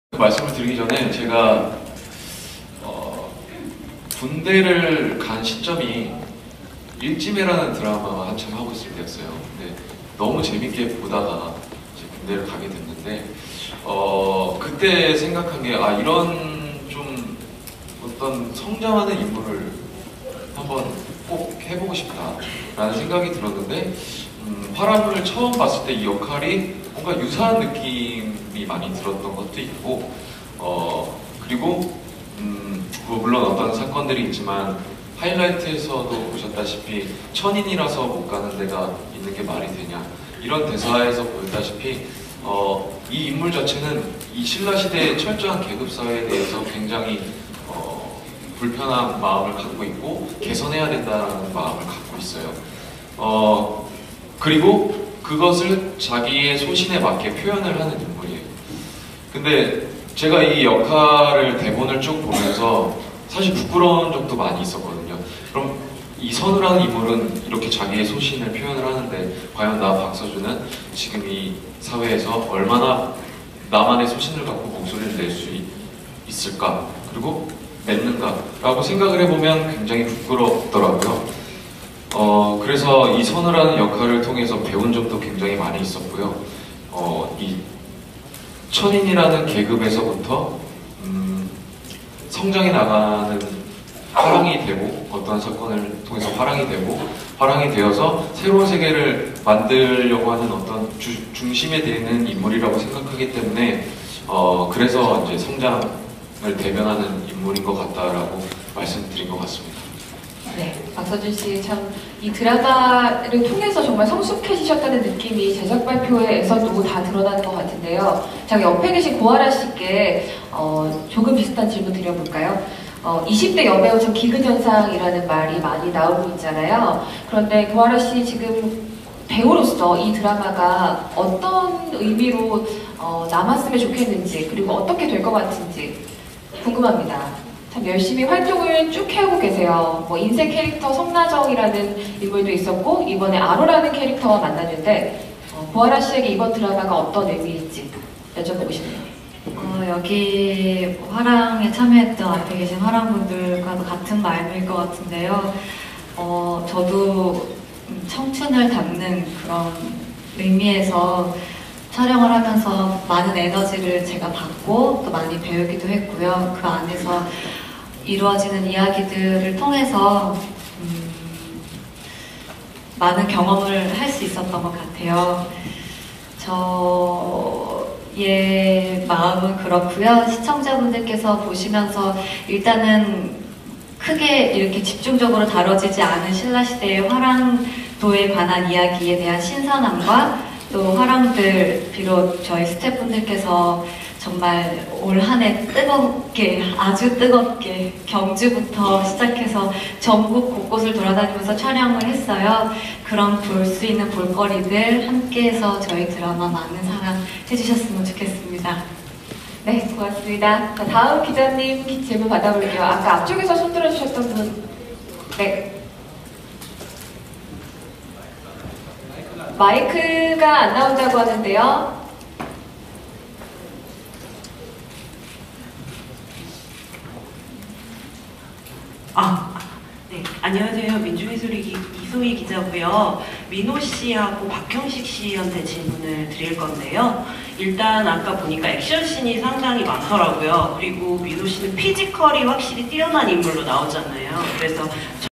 박서준 "일지매 보면서.. 성장드라마 꼭 해보고 싶었다" [드라마] 화랑 제작발표회 (2016.12.16)
2016년 12월 16일(금) 오후 3시, 서울시 강남구 논현동 임피리얼 팰리스 서울 7층 셀레나홀에서 열린 KBS 2TV 새 월화드라마 '화랑'(극본 박은영, 연출 윤성식)의 제작발표회. 박서준, 고아라, 박형식, 최민호, 도지한, 조윤우, 김태형(방탄소년단 뷔) 등이 참석했다. [박재환@KBS미디어]